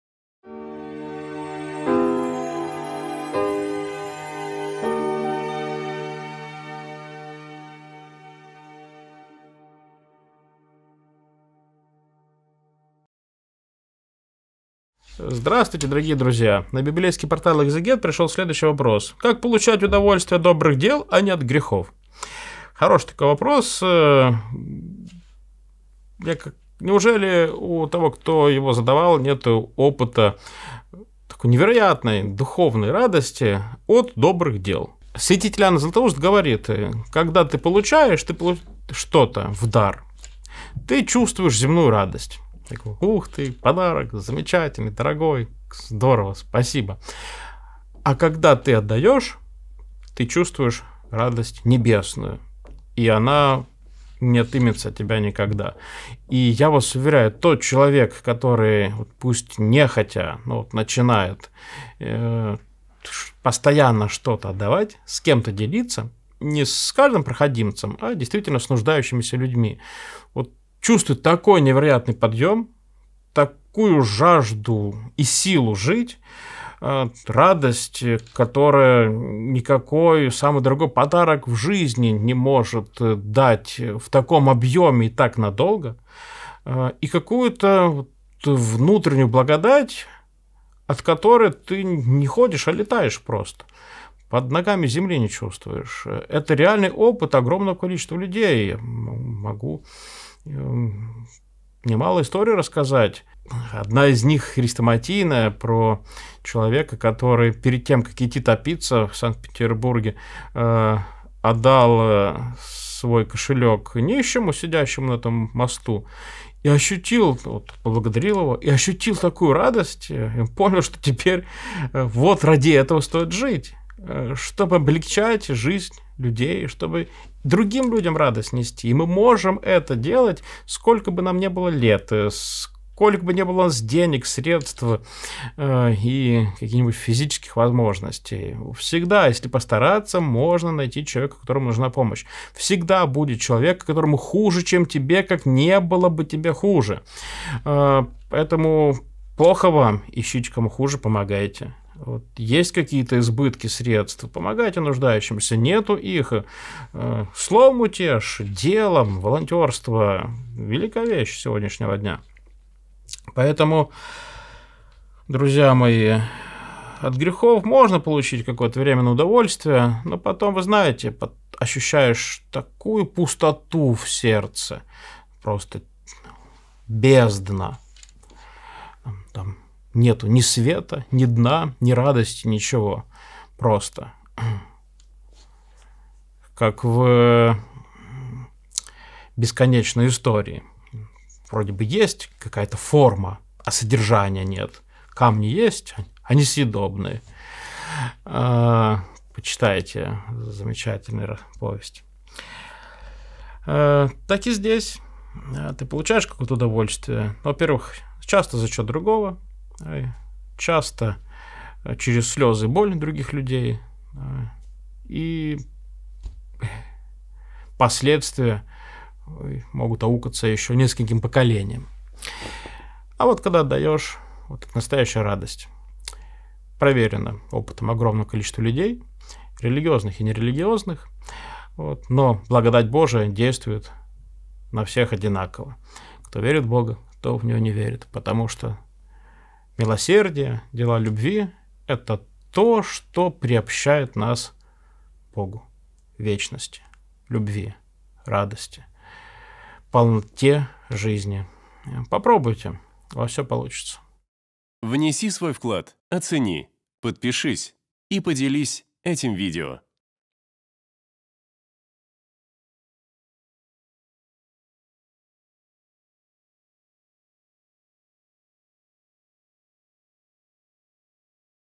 Как получать удовольствие от добрых дел, а не от грехов ? Священник Владислав Береговой
Команда Экзегет.

На библейском портале Экзегет вместе со священником Владиславом Береговым отвечаем на вопрос: "Как получать удовольствие от добрых дел, а не от грехов?"

🔸  Радость Небесная
🔸  Помощь ближнему
🔸  Милосердие
🔸  Благодать Божия